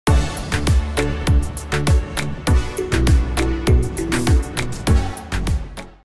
Nespresso - ¡Refresca tu verano con Barista Creations for Ice! | CL
Disfruta estos 2 exquisitos cafés especialmente diseñados para tomarse con hielo en los calurosos días de verano